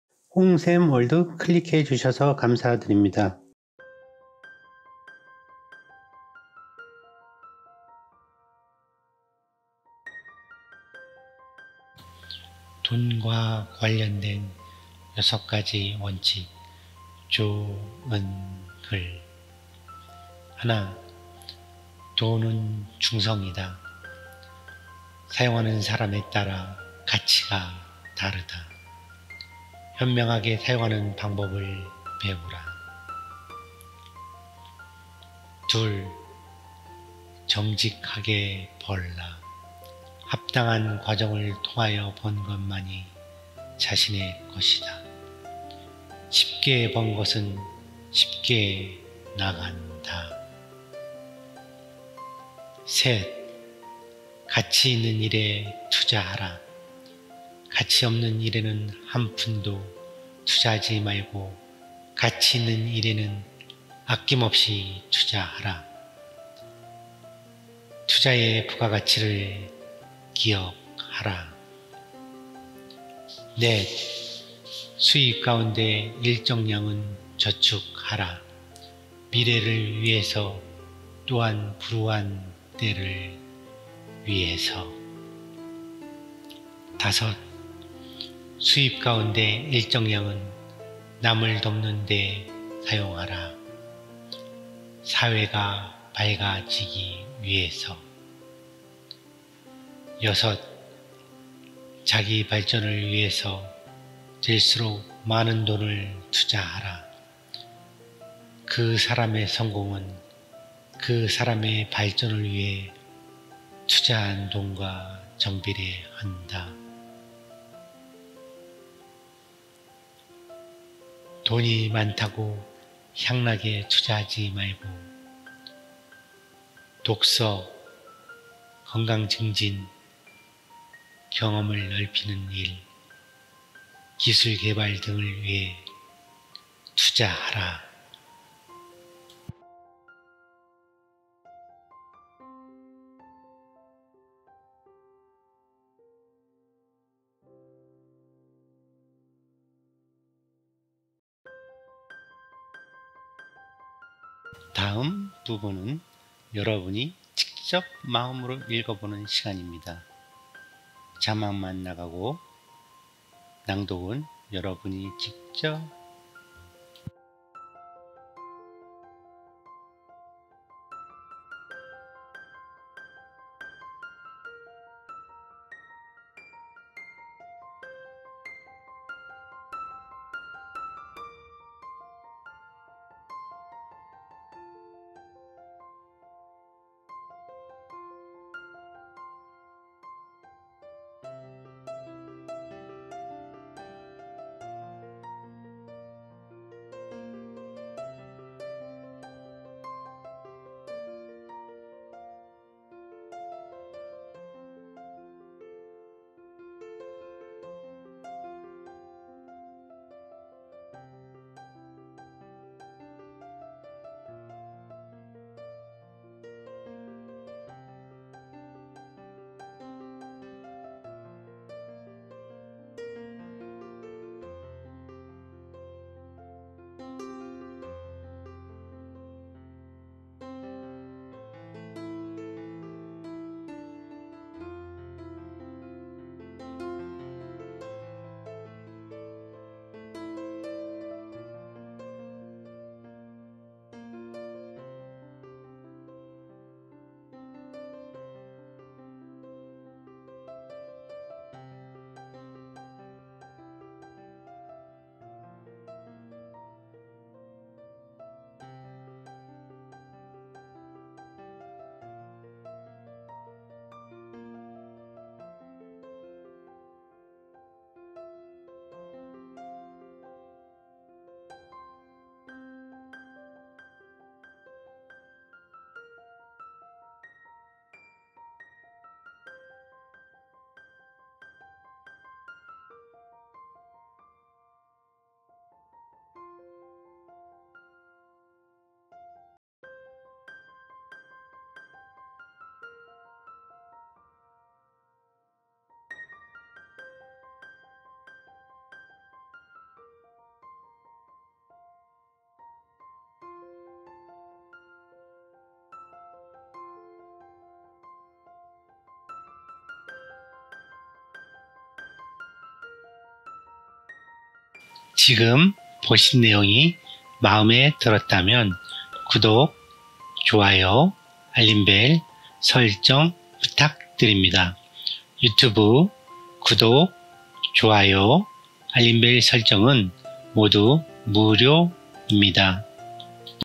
좋은글, + 돈과 관련된 6가지 원칙, 돈은 중성이다, 정직하게 벌라, 자기 발전을 위해서 될수록 많은 돈을 투자하라
좋은글,
돈과 관련된 6가지 원칙,
돈은 중성이다,
정직하게 벌라,
자기 발전을 위해서 될수록 많은 돈을 투자하라

+ 돈과 관련된 6가지 원칙 / 좋은글

1.돈은 중성이다.
사용하는 사람에 따라 가치가 다르다.
현명하게 사용하는 방법을 배우라.

2.정직하게 벌라.
합당한 과정을 통하여 번 것만이 자신의 것이다.
쉽게 번 것은 쉽게 나간다.

3.가치 있는 일에 투자하라.
가치 없는 일에는 한 푼도
투자하지 말고 가치 있는
일에는 아낌없이 투자하라.
투자의 부가가치를 기억하라.

4.수입 가운데 일정량은 저축하라.
미래를 위해서, 또한 불우한 때를 위해.

5.수입 가운데 일정량은 남을 돕는 데에 사용하라.
사회가 밝아지기 위해서.

6.자기 발전을 위해서 될수록 많은 돈을 투자하라.
그 사람의 성공은 그 사람의
발전을 위해 투자한 돈과 정비례한다.
돈이 많다고 향락에 투자하지 말고 독서,
건강 증진, 경험을 넓히는 일,
기술개발 등을 위해 투자하라.